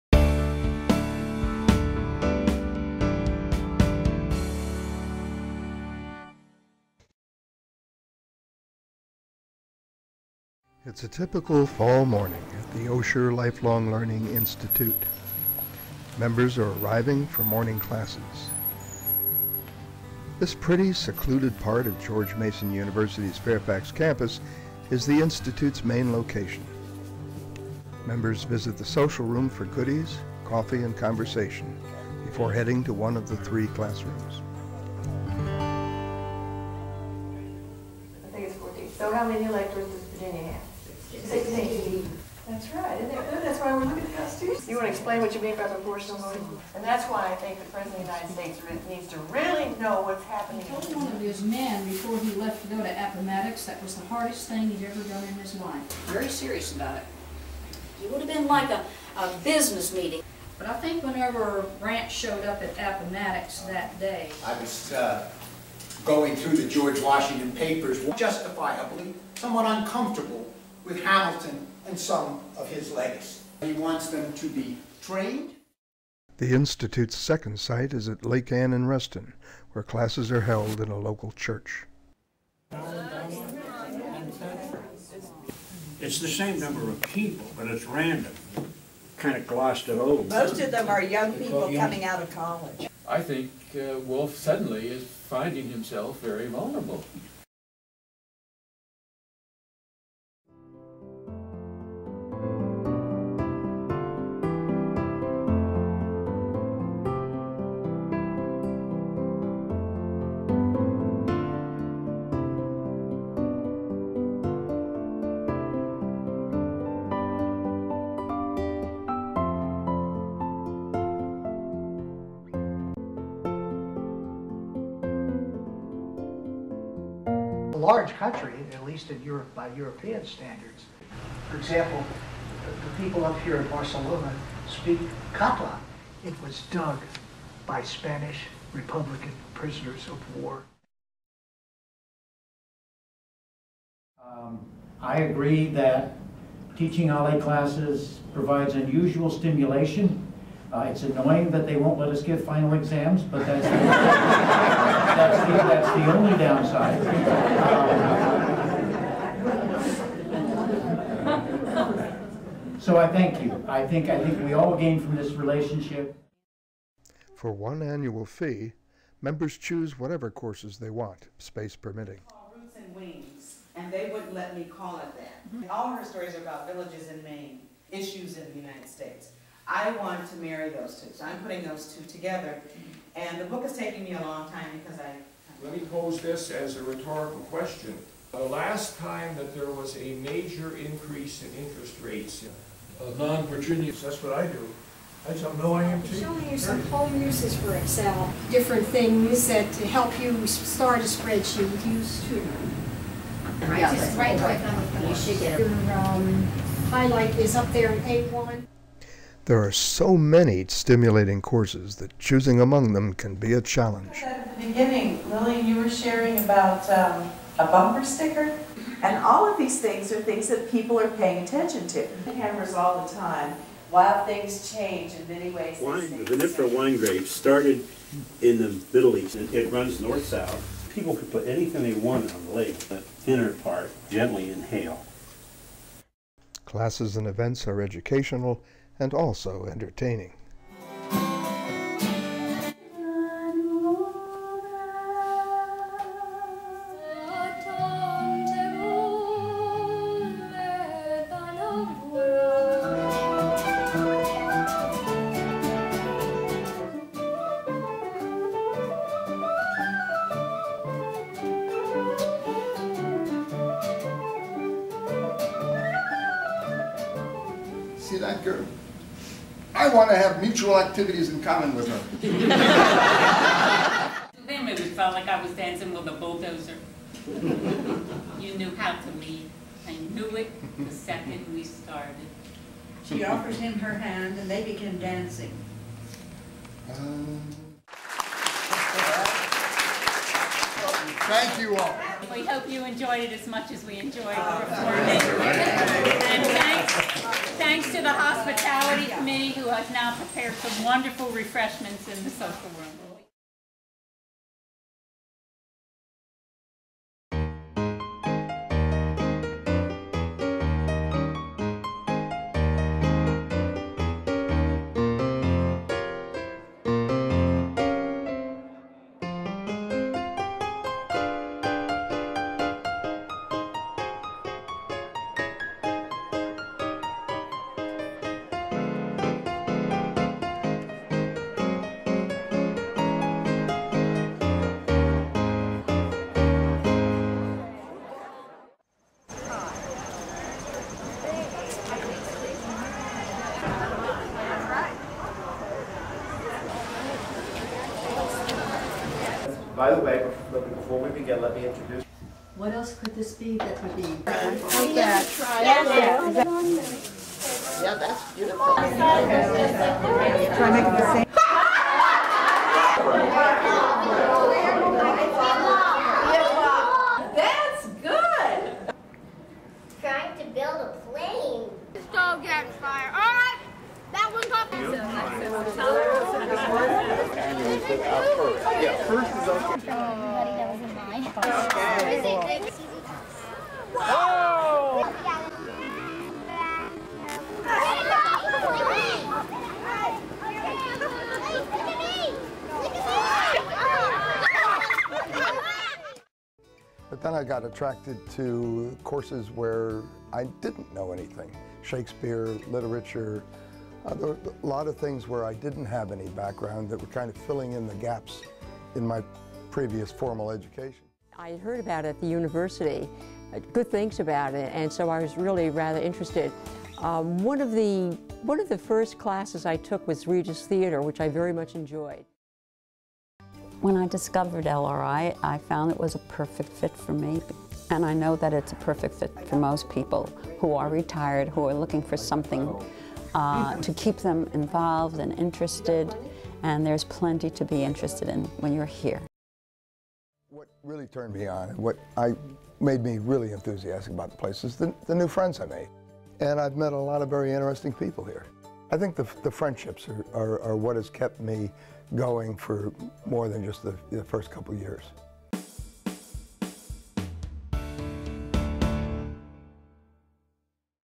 The Story of OLLI at George Mason University
This movie provides an overview of the Osher Lifelong Learning Institute at George Mason University, Fairfax, Virginia. With more than 1,100 members, OLLI-Mason is one of the premier lifelong-learning institutes in the United States, offering numerous daytime courses as well as social and cultural opportunities during four terms each year at three sites in Northern Virginia (Fairfax, Reston and Sterling).